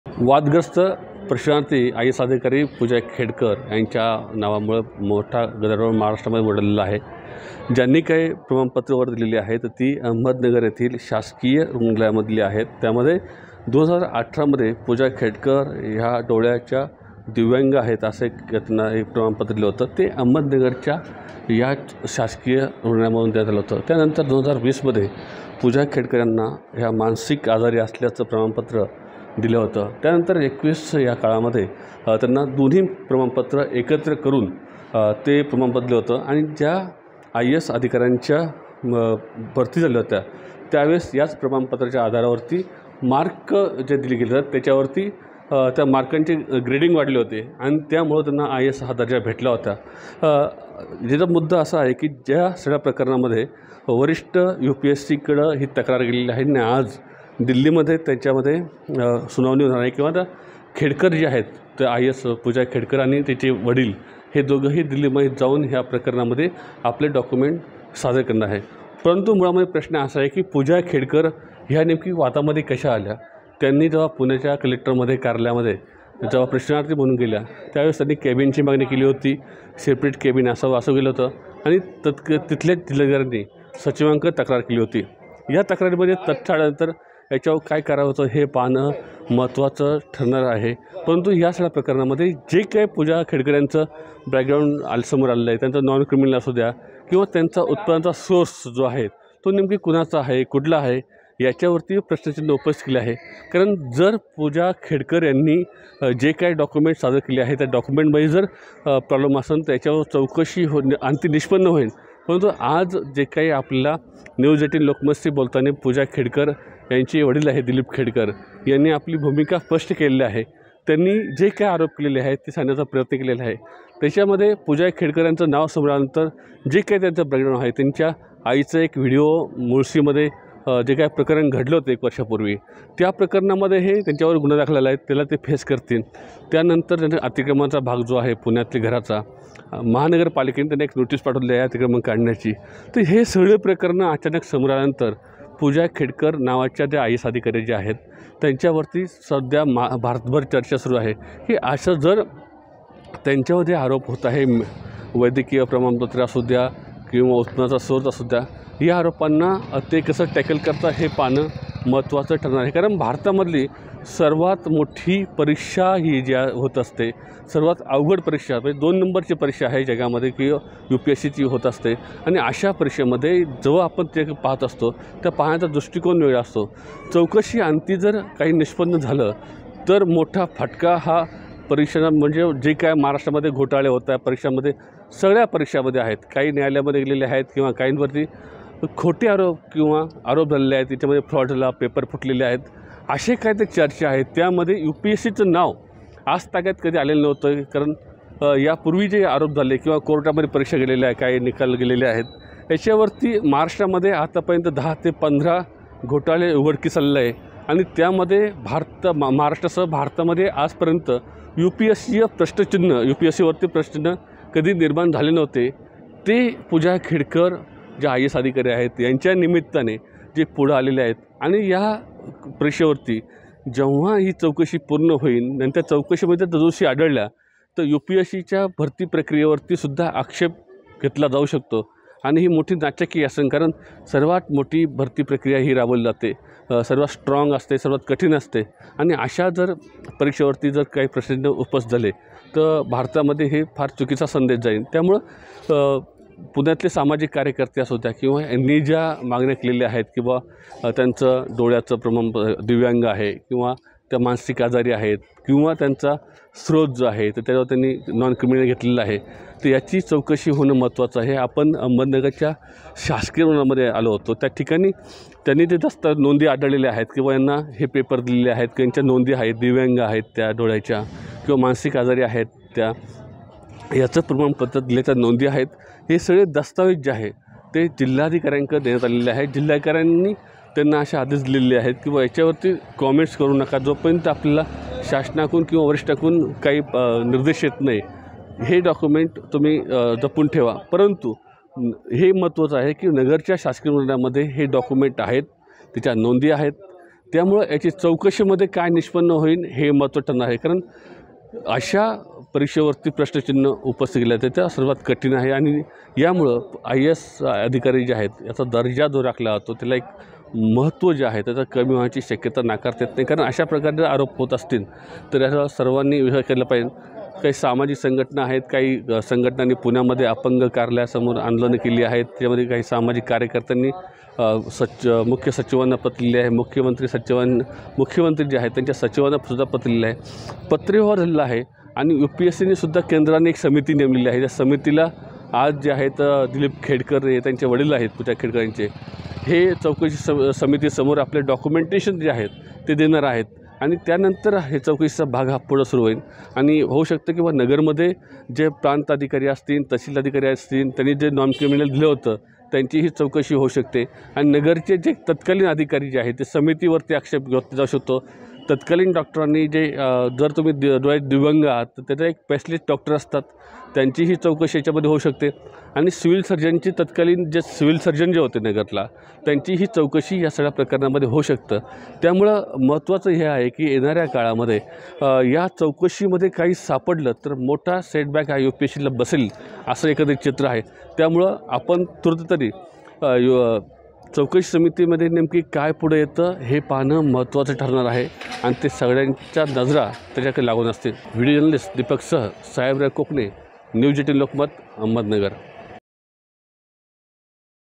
Pooja Khedkar Case Update : पूजाला खोटं प्रमाणपत्र दिलेल्या, नगरच्या रुग्णालयाची पोलखोल
अहमदनगर - वादग्रस्त आयएएस अधिकारी पूजा खेडकर यांना अहमदनगर येथील शासकीय रुग्णालयातून डोळ्याच्या दिव्यांग आणि मानसिक आजार याचे प्रमाणपत्र देण्यात आले होते
Pooja was given a fake certificate, the city hospital's pollhole
AHMEDNAGAR - Controversial IAS officer Pooja Khedkar was given a certificate of eye disability and mental illness from the government hospital in Ahmednagar.